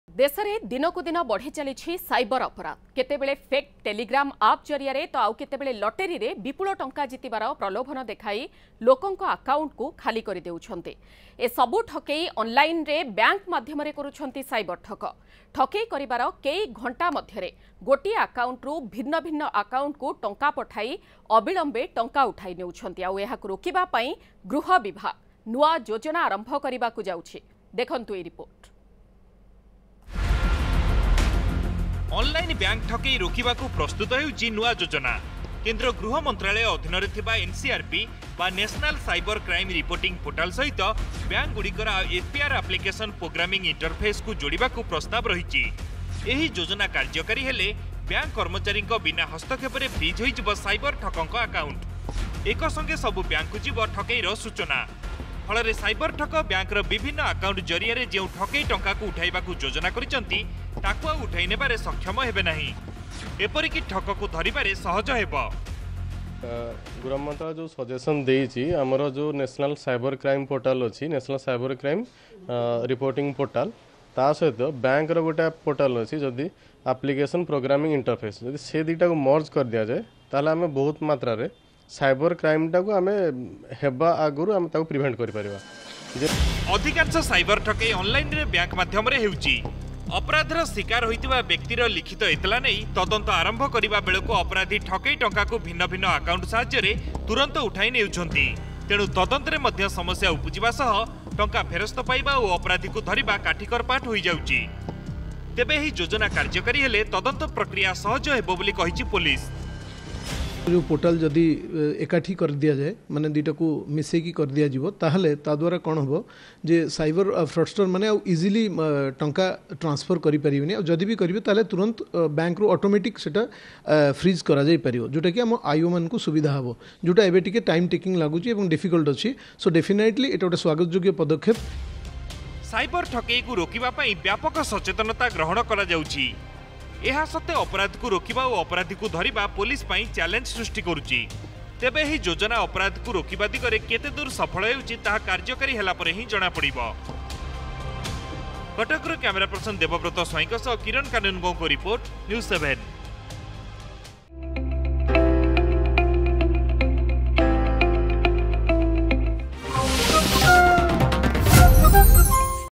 New scheme to prevent online fraud, proposal to link bank's APR interface with NCRP | PrameyaNews7
New scheme to prevent online fraud, proposal to link bank's APR interface with NCRP
ଅନଲାଇନ ଠକେଇ ରୋକିବାକୁ ନୂଆ ଯୋଜନା, NCRP ସହ ବ୍ୟାଙ୍କର ଏପିଆର ଇଣ୍ଟରଫେସ ଯୋଡିବାକୁ ପ୍ରସ୍ତାବ.